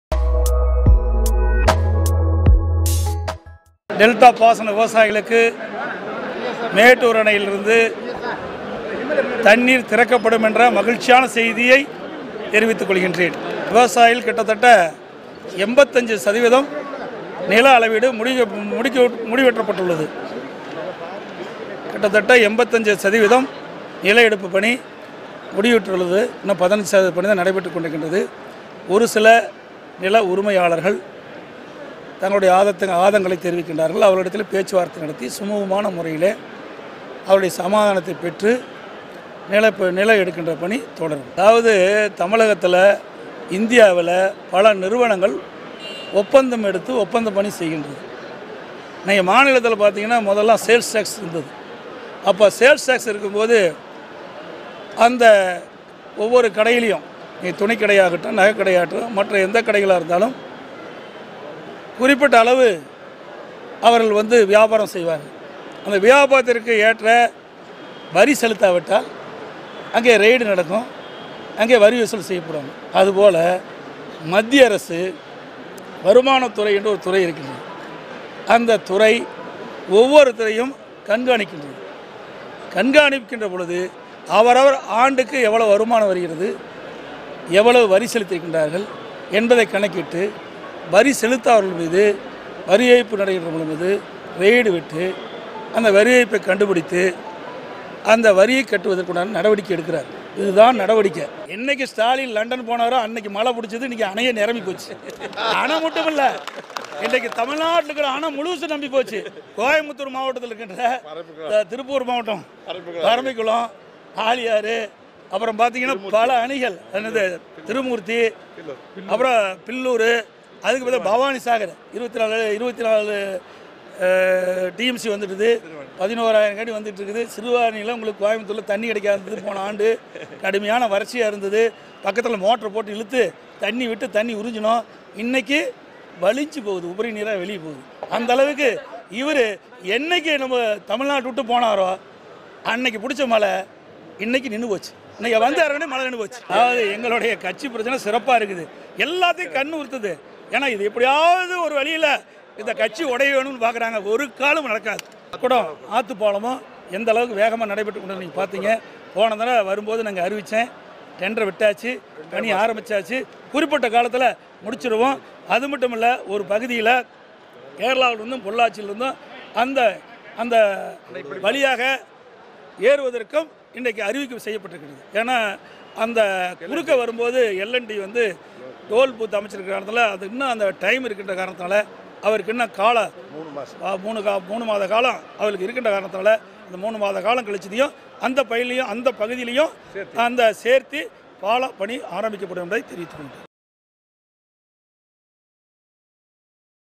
No chance of irregularities in tender applications as it is online- Edapadi
Tamil Nadu CM Edapadi Palanisamy  who was in Coimbatore airport to board a flight to Salem, spoke about the eight lane road and said that 85% of land acquisition was completed and were given of free will.In response to queries about irregularities in tender applications, he said it was not possible as they were online.